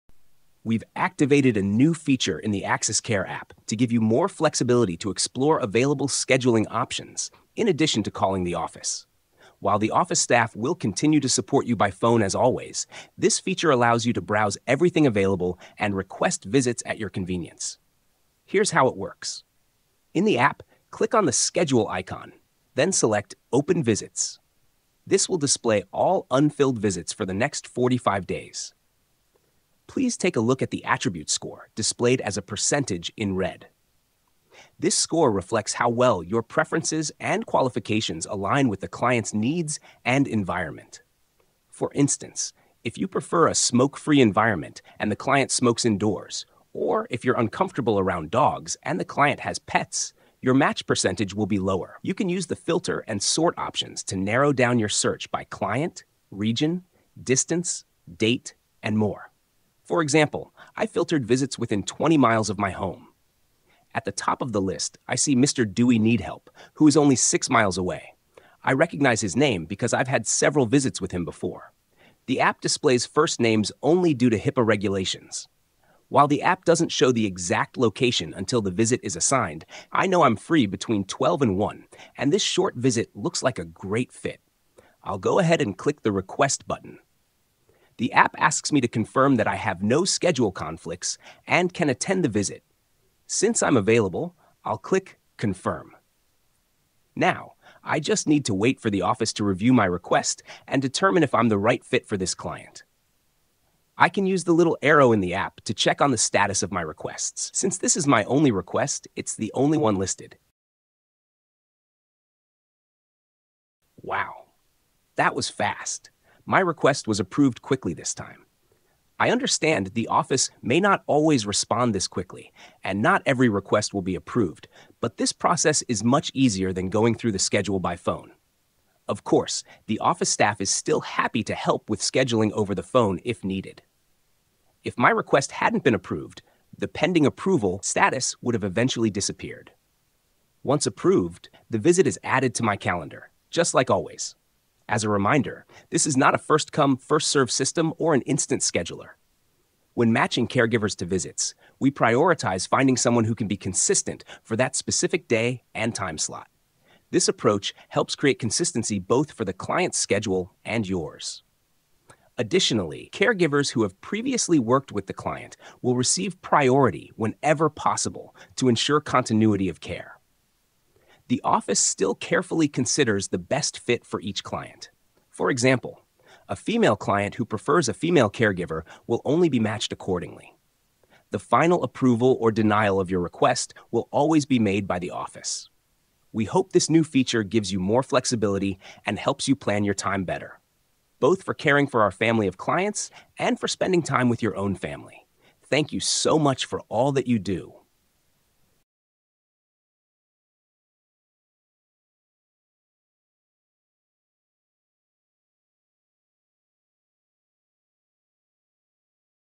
AxisCare Scheduling Tutorial
What you need to get scheduling on the AxisCare App!